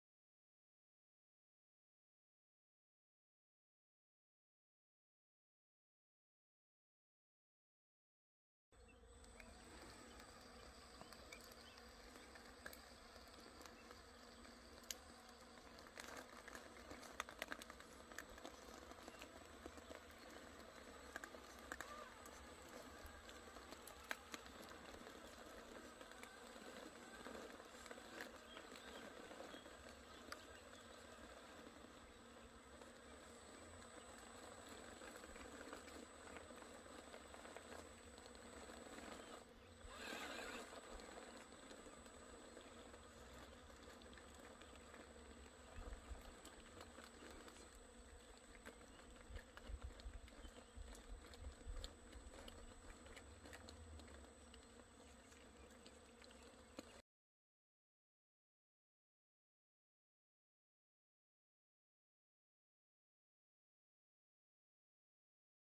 Traxxas TRX4. The impossible climb!!!
Test vertical climb in tree.
The weight of TRX4 is 3780 grams, it has original tires, extra weight in front wheels and the clockweight.